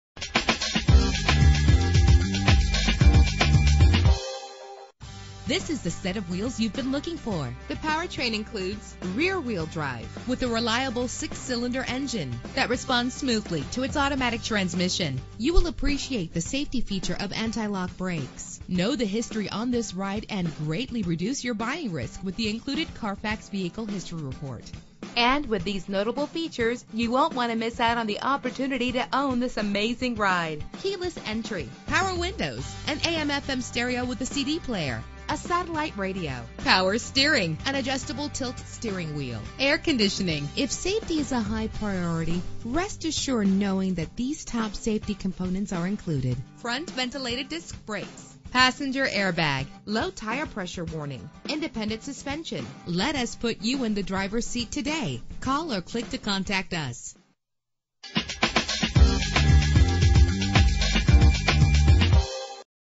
2008 Dodge Ram 1500 - Bedford OH
Year: 2008
Make: Dodge
Model: Ram 1500
Trim: ST/SXT
Engine: 3.7 liter 6 cylinder 12 valve
Transmission: 4-Speed Automatic with VLP
Color: Brilliant Black Crystal Pearlcoat
Mileage: 53473
Address:
123 Broadway Avenue